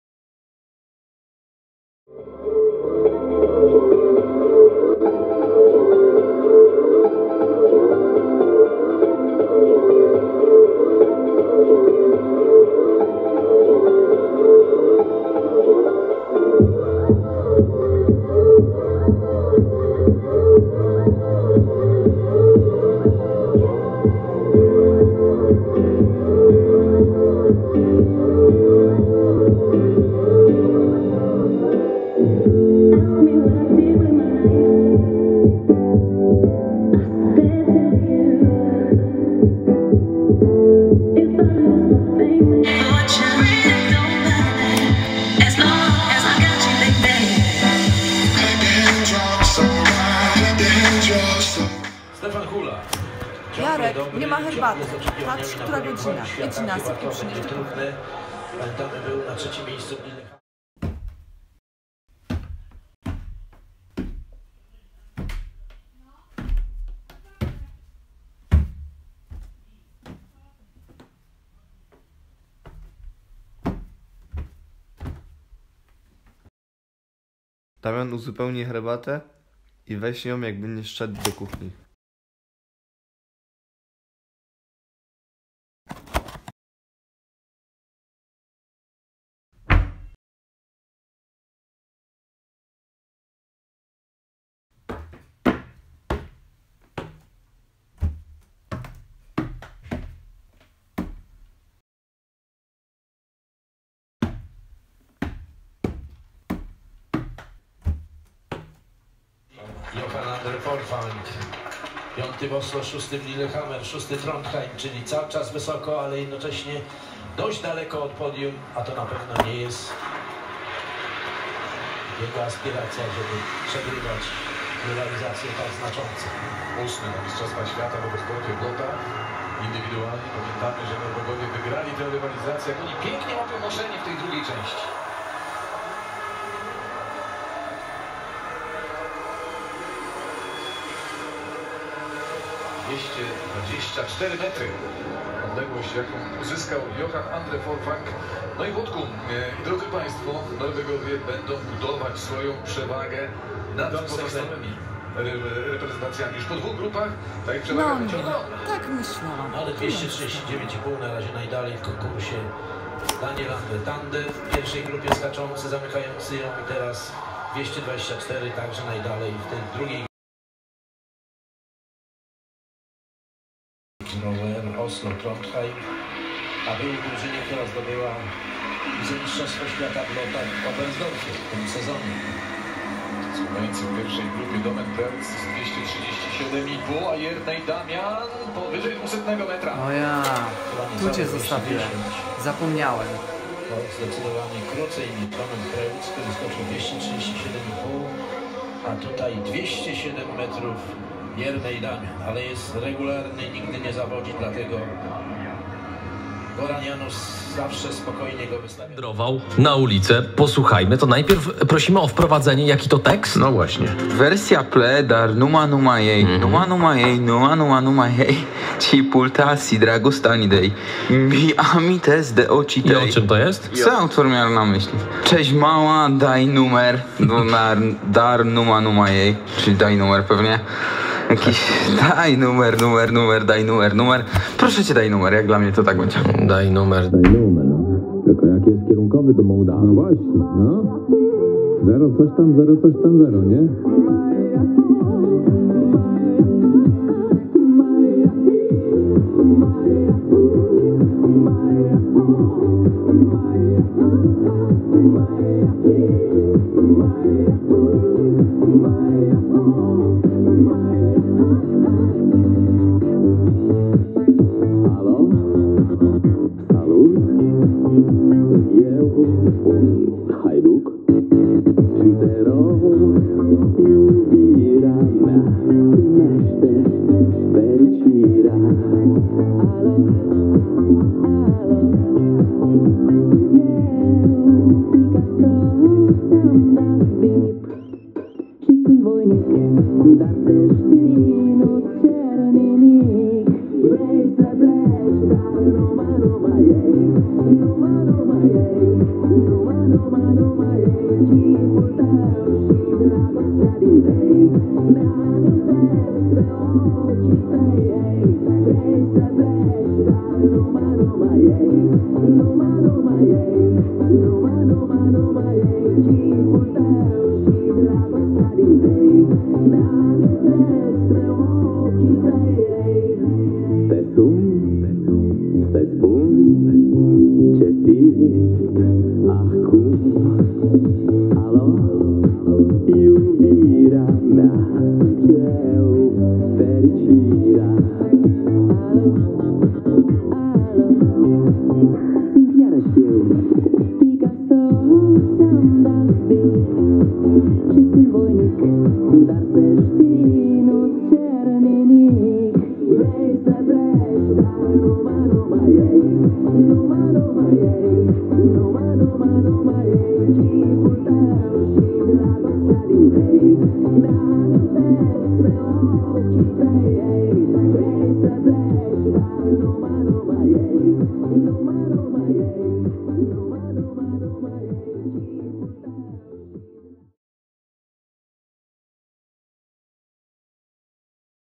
Damian Sobecki - four-channel video
Damian Sobecki, Story of a tea box, [6:21], 2018, four-channel video, Academy of Fine Artsin Cracow; Studio of Recording and Processing, topic of exercise: ‘’Make short four-channel video. Timing is very important in this homework, first of all you must have a plan with time, and transitions between every channel’’.